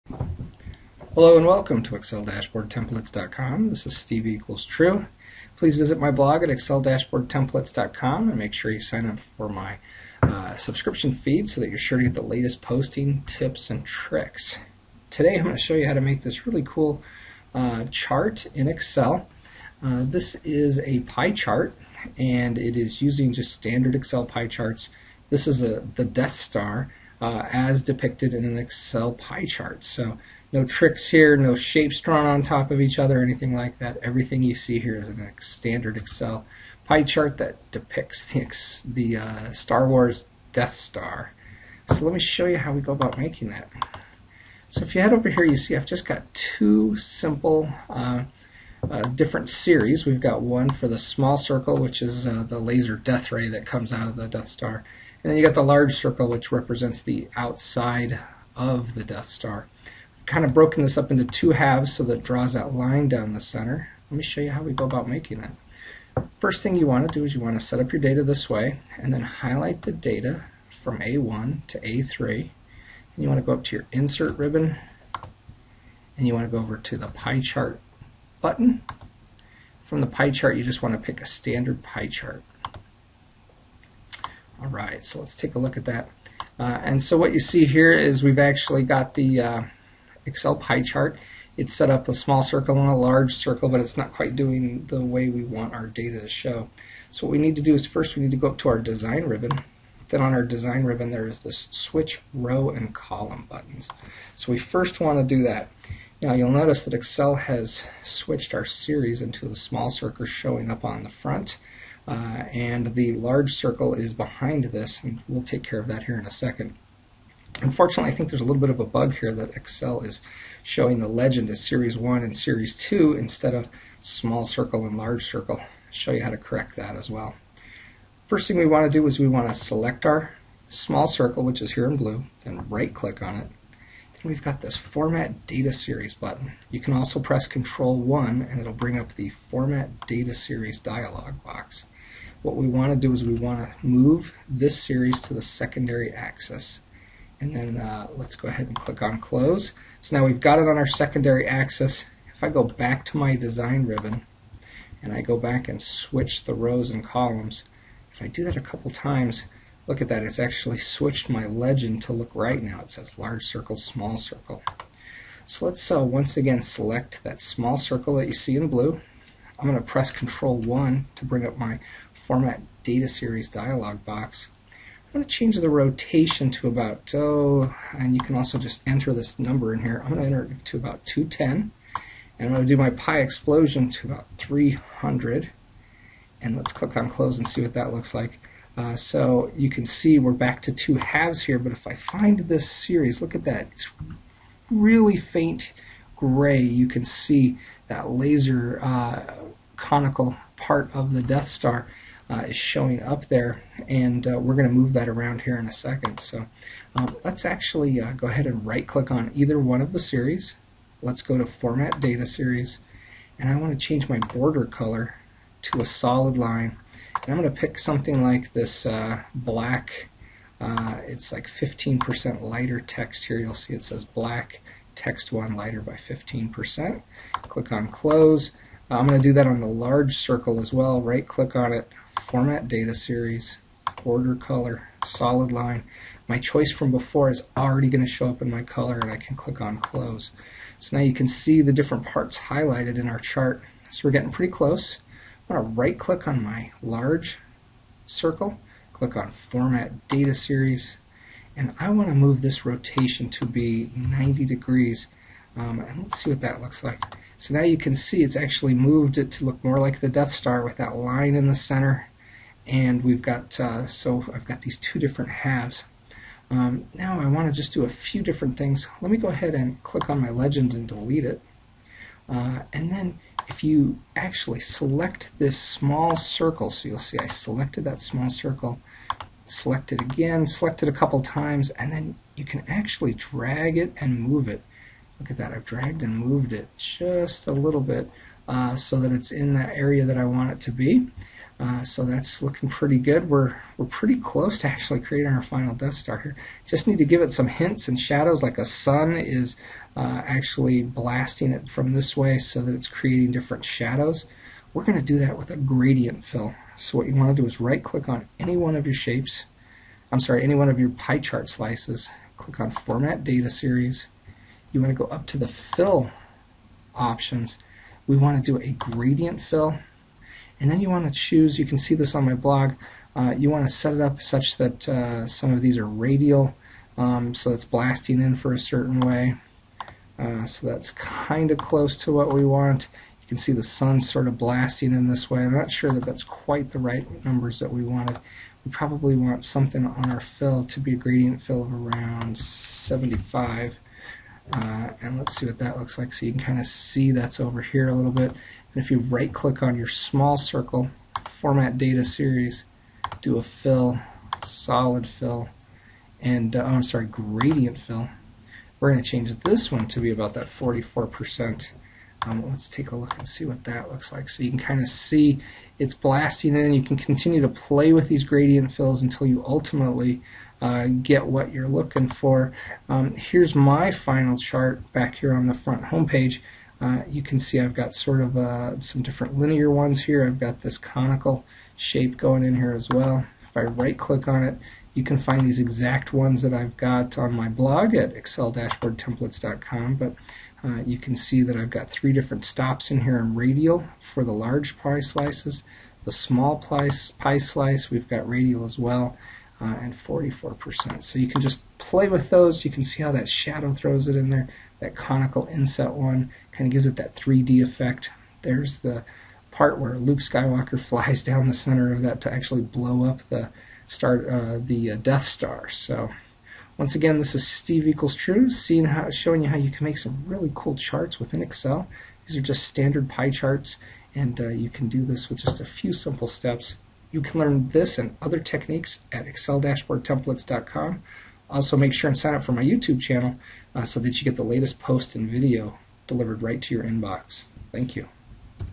How-to Make an Excel Death Star Pie Chart
Ever wondered if you could make a Death Star Chart in Excel only using pie charts?  Let me show you how to use pie charts to make your own Death Star Graph.